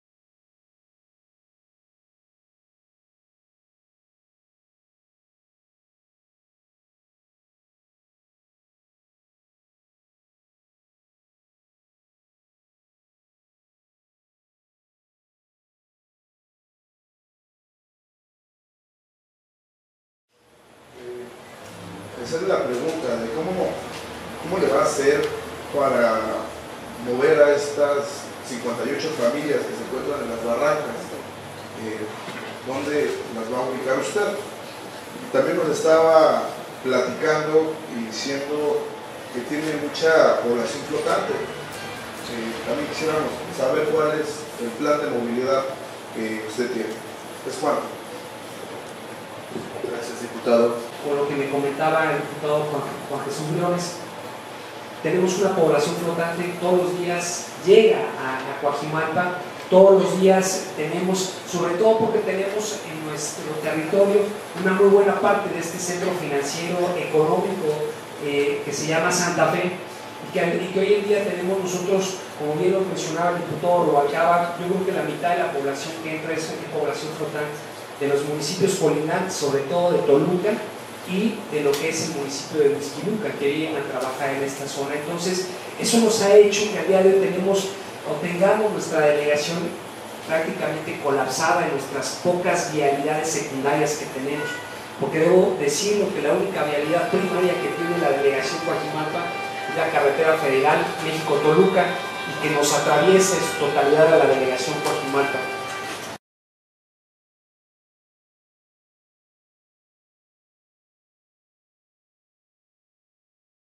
Dip. Juan Briones de morena cuestiona a delegado de Cuajimalpa
Preguntas del diputado Juan Briones durante la Reunión de Trabajo del Presupuesto 2016 con el Delegado de Cuajimalpa Miguel Ángel Salazar Martínez.